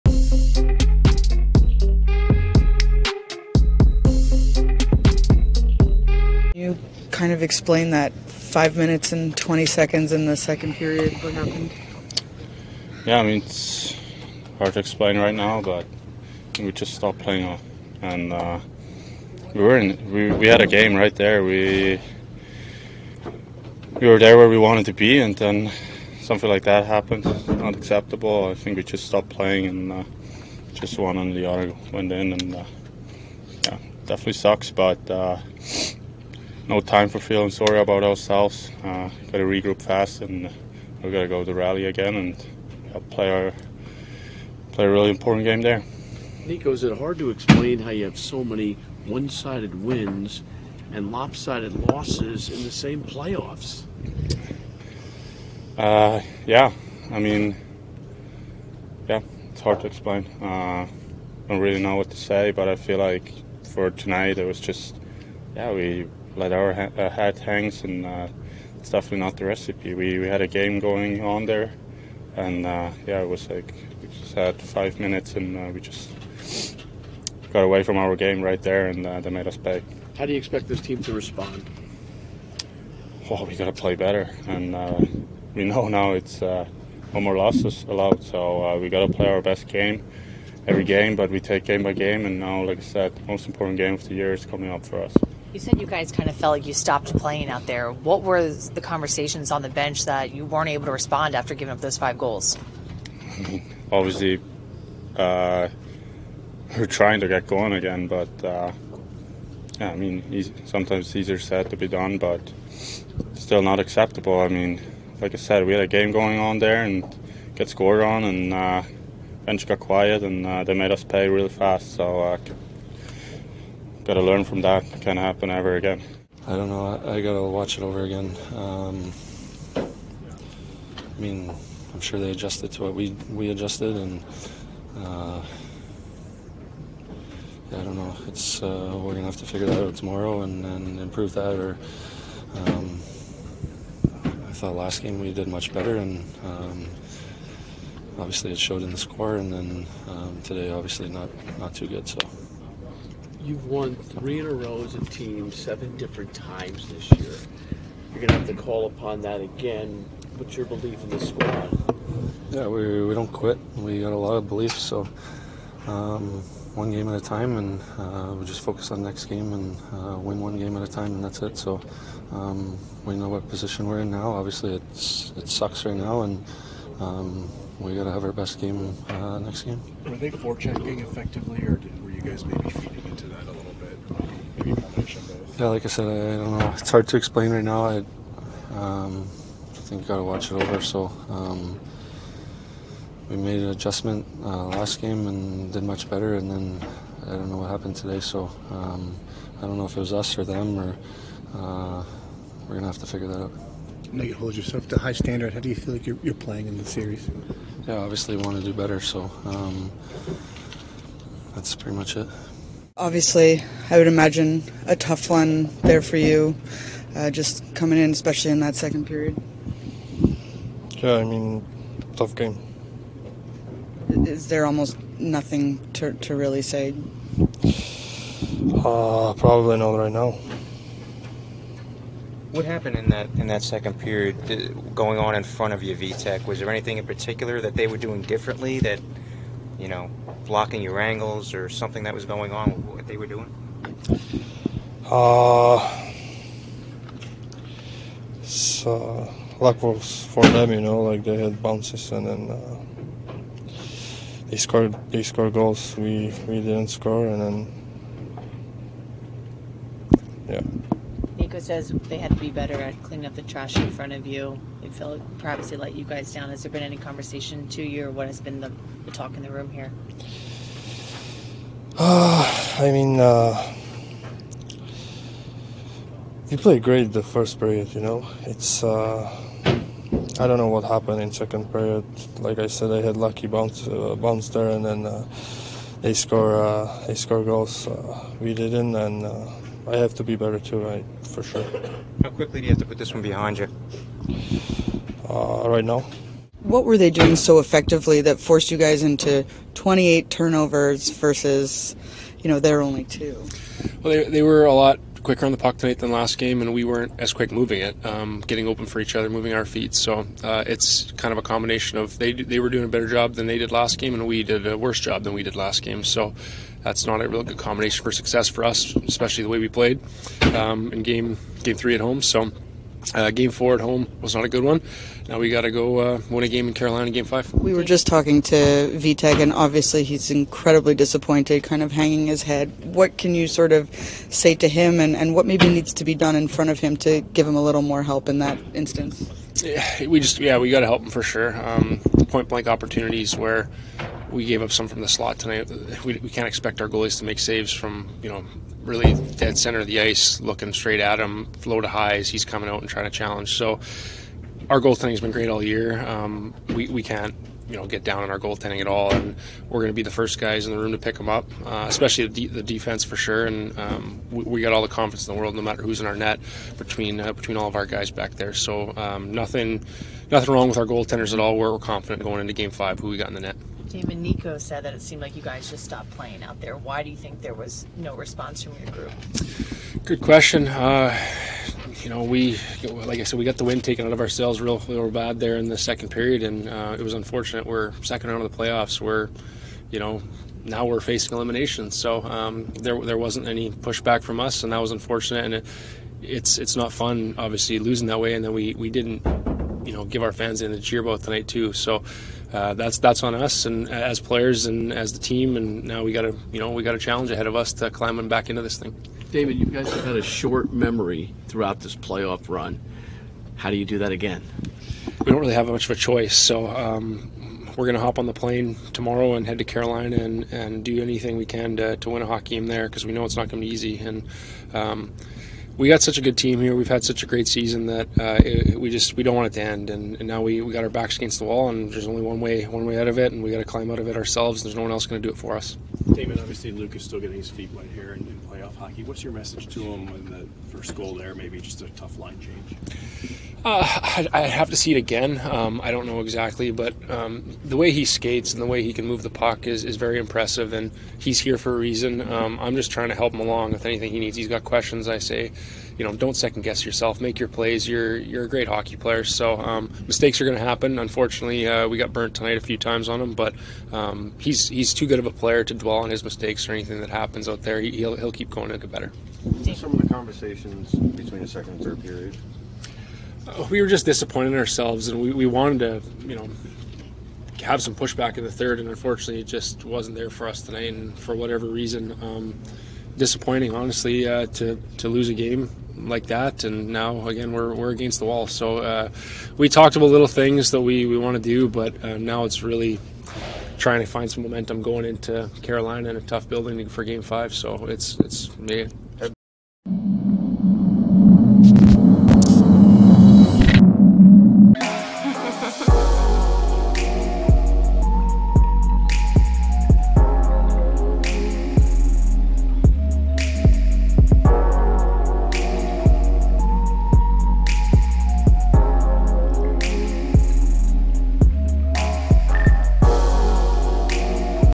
Nico Hischier, Dougie Hamilton, Vanecek & Severson React to Devils Falling Down 3-1 vs. Carolina
Watch as Nico Hischier, Dougie Hamilton, Vitek Vanecek & Severson React to Devils Falling Down 3-1 vs. Carolina